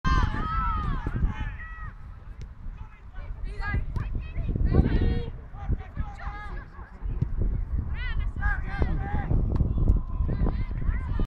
Under 19, 4^ Rit (anticipo): Parma-San Marino, 15' st tiro distanza Baccanti, parato
- Juniores Under 19, 4^ Giornata del Girone di Ritorno (anticipo): Parma-San Marino, 15' st tiro dalla distanza di Marta Baccanti, parato
Campo in sintetico “Quirino Zanichelli” del Complesso Polisportivo Stuard “Ferruccio Bellè”, San Pancrazio, PR, giovedì 7 Marzo 2019